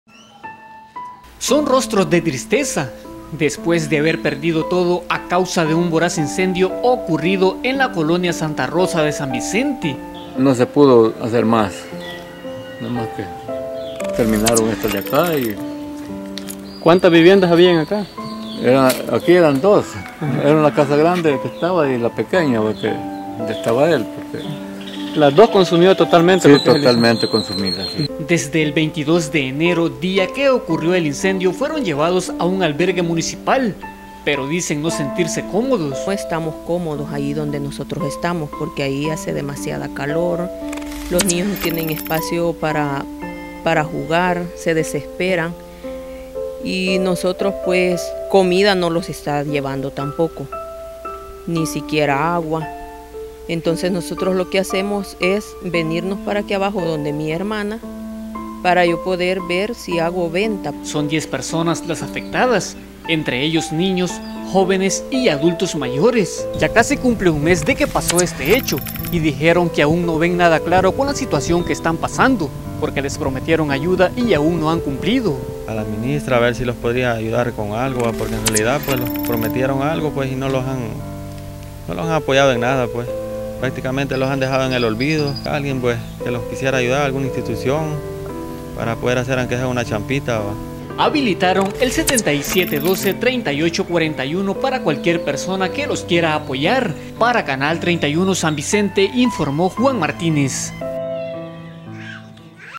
NOTICIA
NOTICIA: Familia que perdió todo a causa de un incendio en la colonia Santa Rosa de San Vicente pide ayuda de personas de buen corazón.-